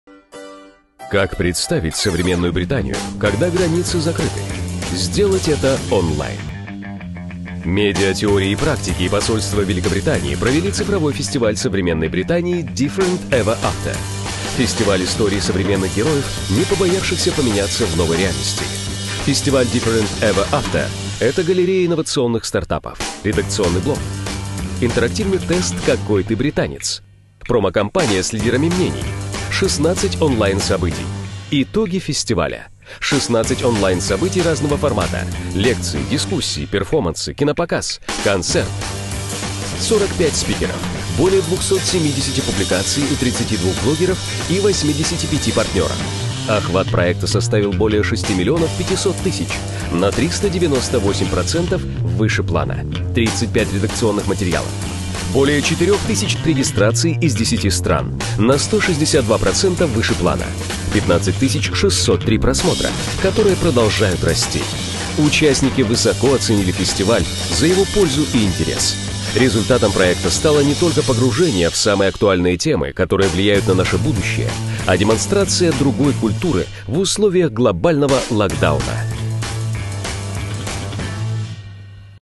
Фестиваль Different Ever After
В рамках сотрудничества Посольства Великобритании в России и T&P состоялся первый в своем роде онлайн-фестиваль современной британской культуры Different Ever After. 16 онлайн-событий объединили более 4000 участников, множество исполнителей музыкальной сессии EKBRUM и 41 спикера, которые обсудили широкий пул вопросов, от выхода Великобритании из ЕС и психологии реакции на климатические изменения до доступного туризма и того, как сделать себя счастливее, помогая другим.